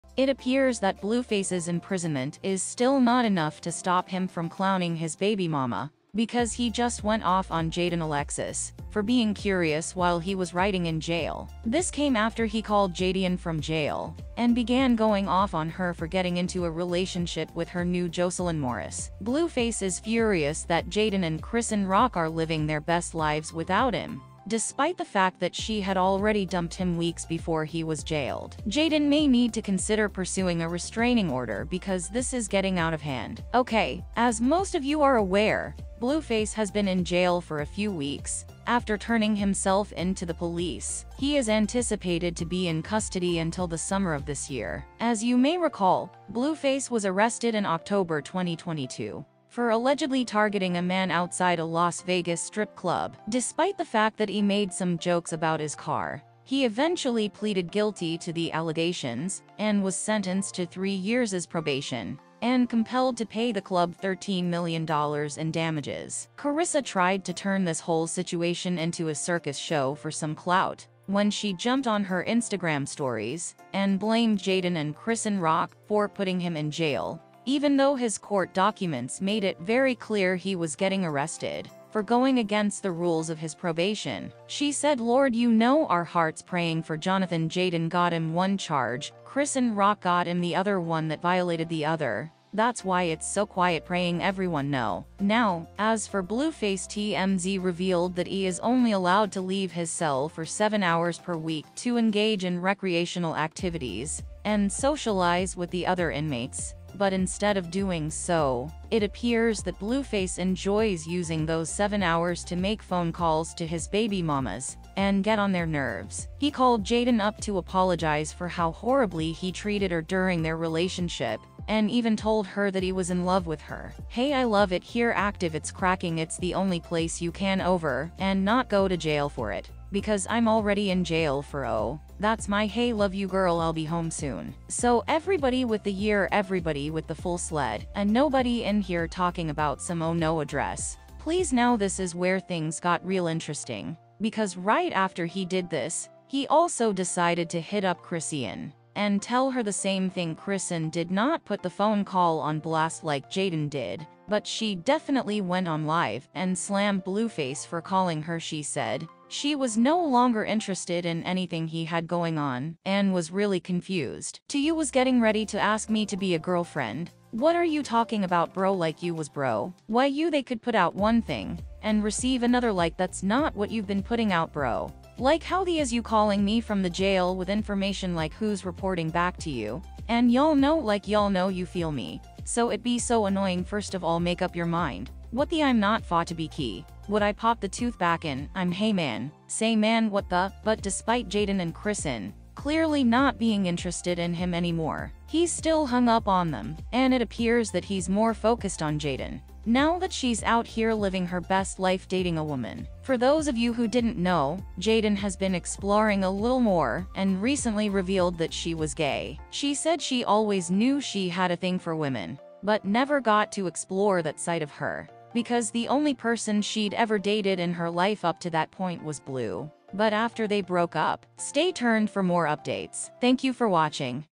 Blueface Blast Jaidyn Alex After He Find Out She's Into Lesbian When He Get Locked Up In Jail
It appears that Blueface's imprisonment is still not enough to stop him from clowning his baby mama because he just went off on Jaiden Alexis for being curious while he was riding in jail. This came after he called Jadien from jail and began going off on her for getting into a relationship with her new Joselyn Morris.